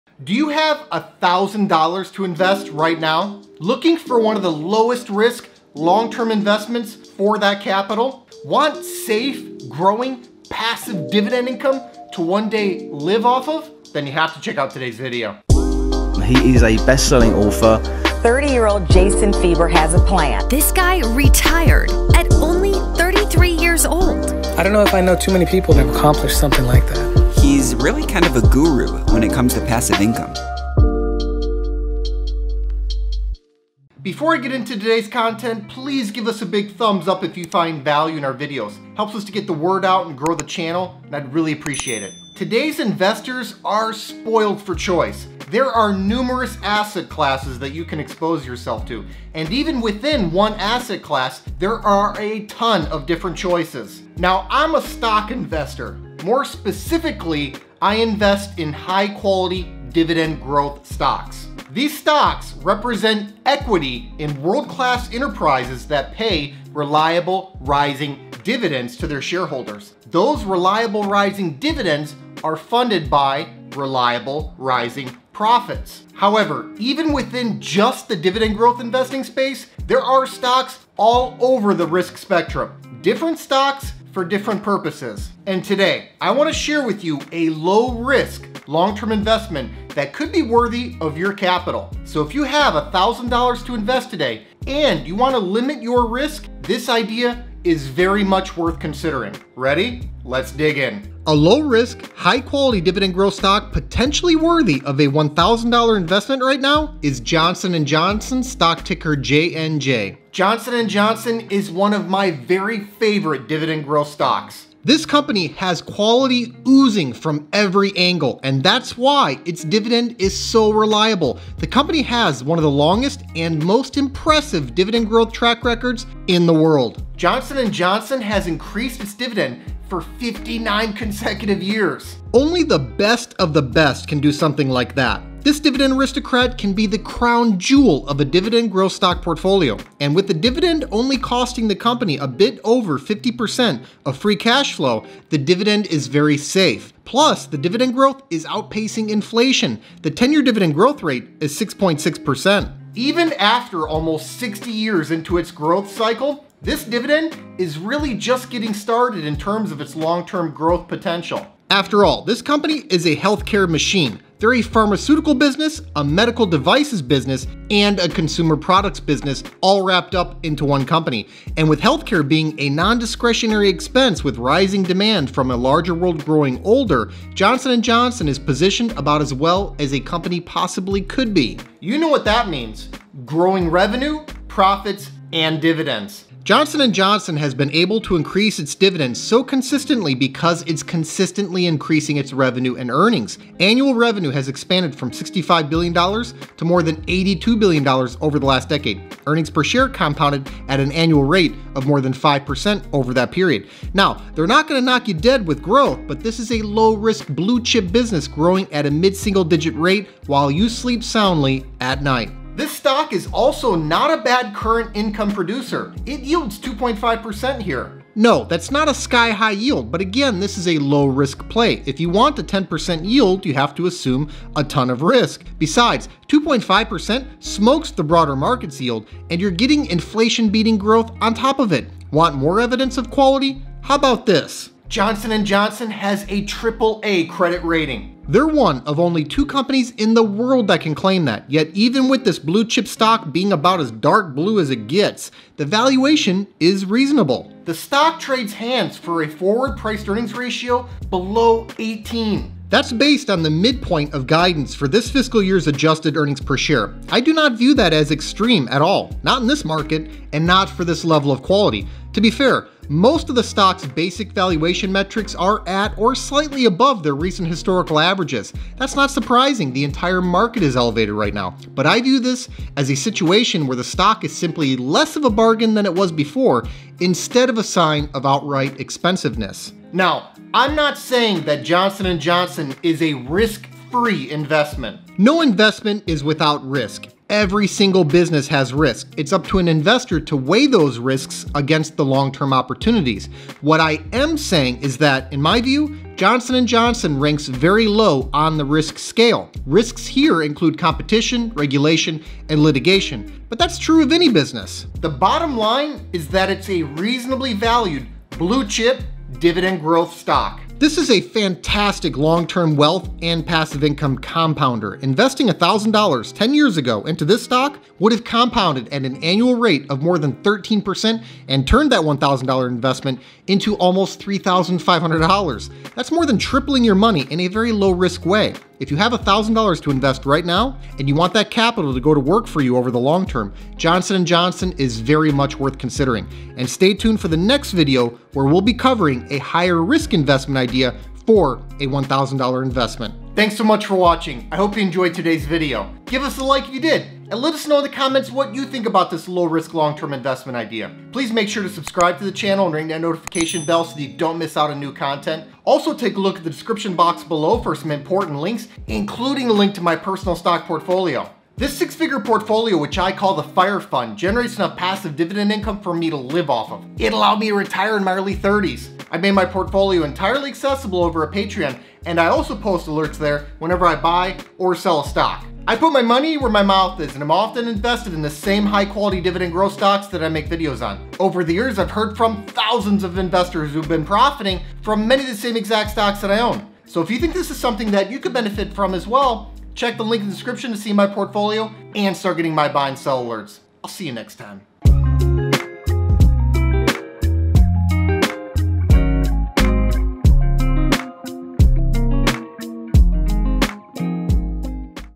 Where to Invest $1,000 Right Now (Safe, Low-Risk Stock)
Today’s investors are spoiled for choice. There are numerous asset classes that you can expose yourself to. And even within just one asset class, there are a ton of choices.

Now, I’m a stock investor. Specifically, I invest in high-quality dividend growth stocks. These stocks represent equity in world-class enterprises that pay reliable, rising dividends to their shareholders. Those reliable, rising dividends are funded by reliable, rising profits.

However, even within just the dividend growth space, there are stocks all over the risk spectrum. Different stocks for different purposes. And today, I want to share with you a low-risk, long-term investment that could be worthy of your capital.

So if you have $1,000 to invest today, and you want to limit your risk, this is definitely an idea for you to consider.

A low-risk, high-quality dividend growth stock potentially worthy of a $1,000 investment right now is Johnson & Johnson (JNJ). Johnson & Johnson is one of my very favorite dividend growth stocks.

This company has quality oozing from every angle. And that’s why its dividend is so reliable. The company has one of the longest and most impressive dividend growth track records in the world.

Johnson & Johnson has increased its dividend for 59 consecutive years.

Even after almost 60 years into a growth cycle, this dividend is really just getting started in terms of its long-term growth potential.

Johnson & Johnson has been able to increase its dividend so consistently because it’s consistently increasing its revenue and earnings. Annual revenue has expanded from $65 billion to more than $82 billion over the last decade. Earnings per share compounded at an annual rate of more than 5% over that period. Now, they’re not going to knock you dead with growth, but this is a low-risk blue-chip business growing at a mid-single-digit rate while you sleep soundly at night.

This stock is also not a bad current income producer – it yields 2.5% here.

No, that’s not a sky-high yield. But again, this is a low-risk play. If you want a 10% yield, you have to assume a ton of risk. Besides, 2.5% smokes the broader market’s yield. And you’re getting inflation-beating growth on top of it. Want more evidence of quality? How about this?

Johnson & Johnson has a AAA credit rating.

They’re one of only two companies in the world that can claim that. Yet even with this blue-chip stock being about as dark blue as it gets, the valuation is reasonable.

The stock trades hands for a forward P/E ratio below 18!

That’s based on the midpoint of guidance for this fiscal year’s adjusted EPS. I do not view that as extreme at all. Not in this market. And not for this level of quality. To be fair, most of the stock’s basic valuation metrics are at or slightly above their recent historical averages. That’s not surprising. The entire market is elevated right now. But I view this as a situation where the stock is simply less of a bargain than it was before instead of a sign of outright expensiveness.

Now, I’m not saying that Johnson & Johnson is a risk-free investment.

No investment is without risk. Every single business has risk. It’s up to an investor to weigh those risks against the long-term opportunities. What I am saying is that, in my view, Johnson & Johnson ranks very low on the risk scale. Risks here include competition, regulation, and litigation. But that’s true of any business.

The bottom line is that it’s a reasonably valued blue-chip dividend growth stock.

This is a fantastic long-term wealth and passive income compounder. Investing $1,000 10 years ago into this stock would have compounded at an annual rate of more than 13% and turned that $1,000 into almost $3,500. That’s more than tripling your money in a very low-risk way.

If you have $1,000 to invest right now, and you want that capital to go to work for you over the long term, Johnson & Johnson is very much worth considering.